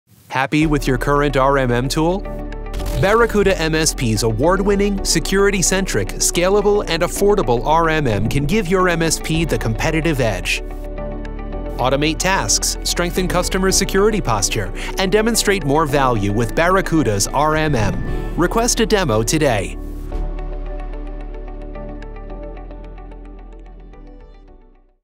Barracuda RMM
The first RMM tool in its class to include a built-in security assessment tool that enables MSPs to quickly assess customers’ networks, deliver multi-layered security, monitor anomalies, and seamlessly recover data in the event of an attack.